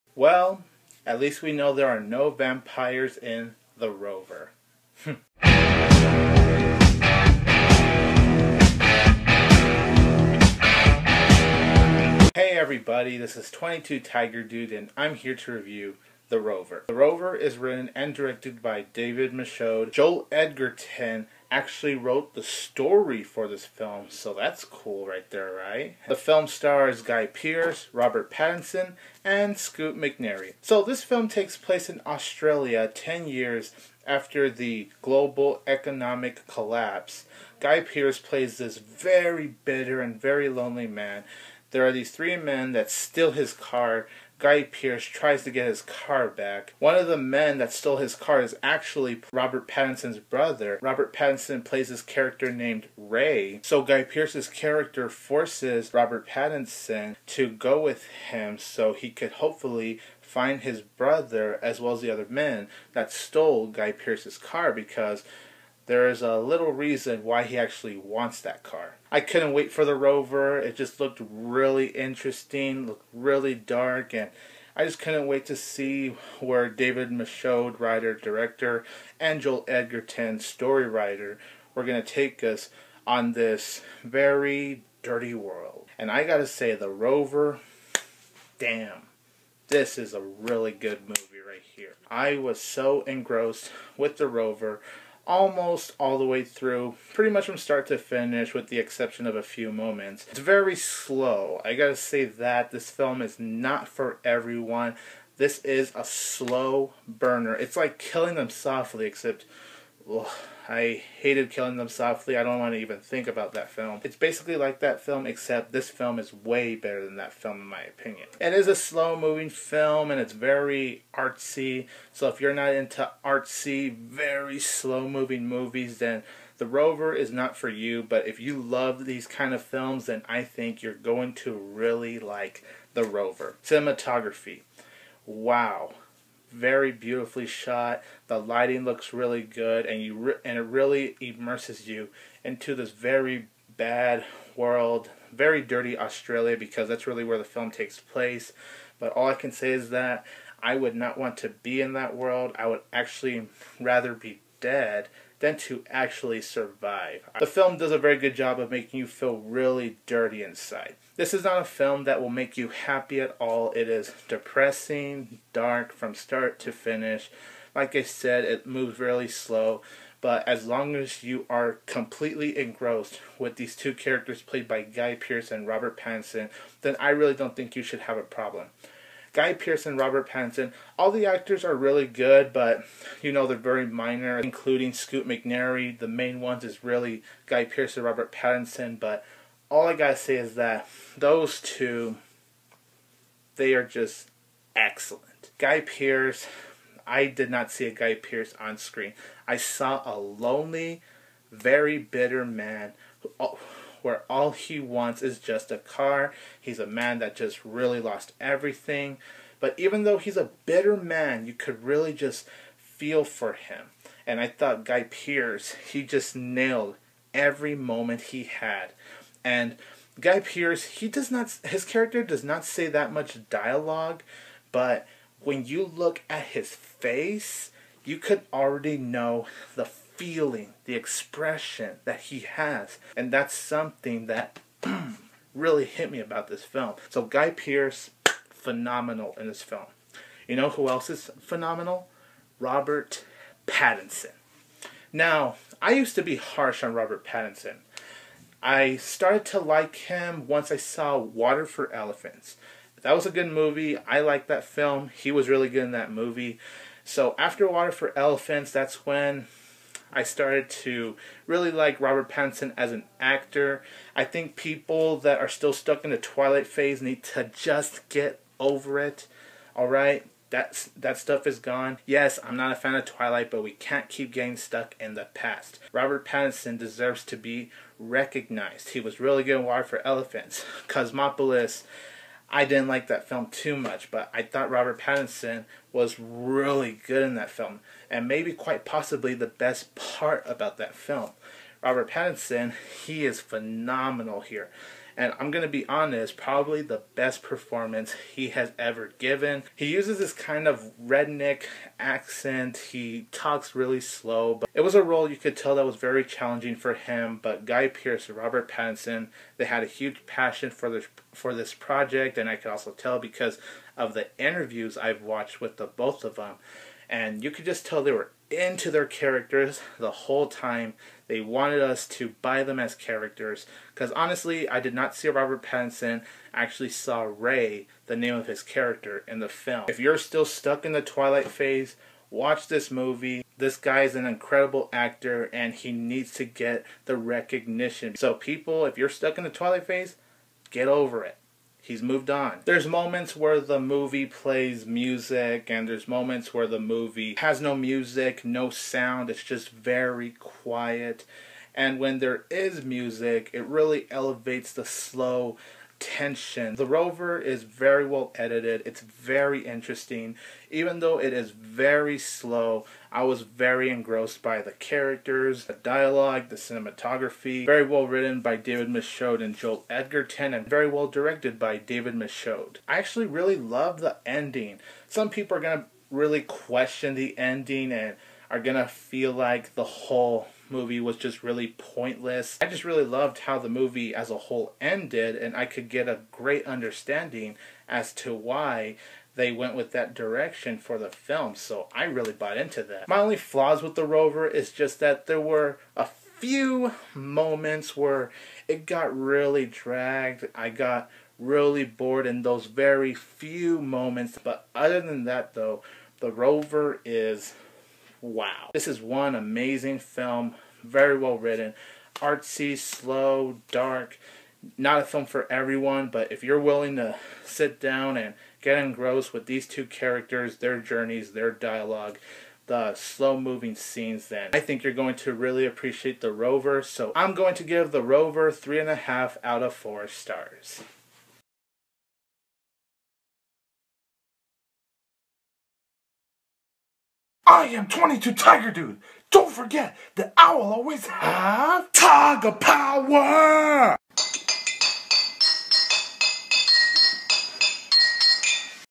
The Rover movie review
22tigerdude reviews the crime drama film written and directed by David Michod. Story by Joel Edgerton. Starring Guy Pearce, Robert Pattinson, and Scoot McNairy. What are his thoughts on this film? find out here.